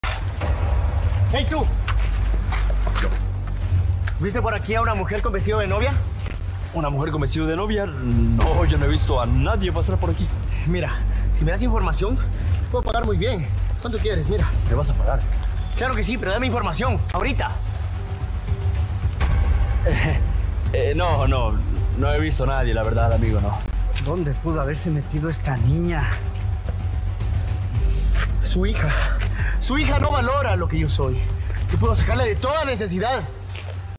Secretos en la Boda de Mireia
Los secretos que esconde Mireia en su boda, casi se matador sufrir acoso, sobornos  chantaje. Pero no sabía que este hombre le salvaría al vida.